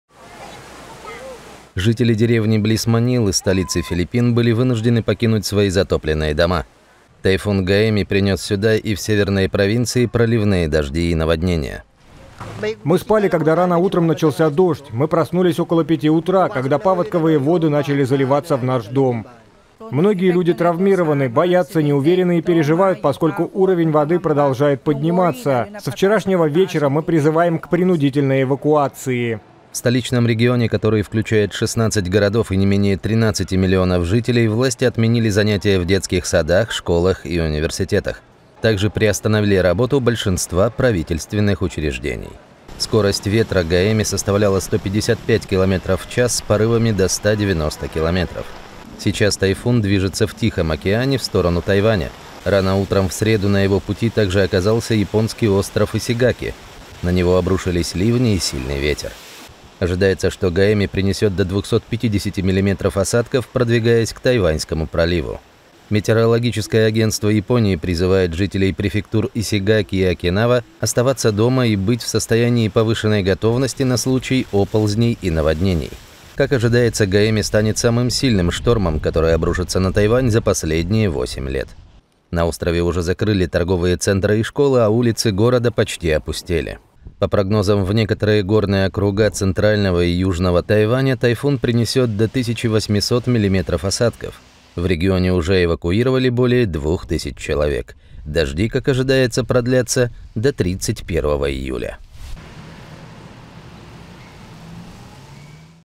Тайфун «Гаэми» дошёл до Филиппин и Японии и готов со всей силой обрушиться на Тайвань
Тайфун «Гаэми» прошёл через регионы Филиппин и Японии и готов со всей силой обрушиться на Тайвань. Для острова он станет самым мощным тайфуном за последние восемь лет.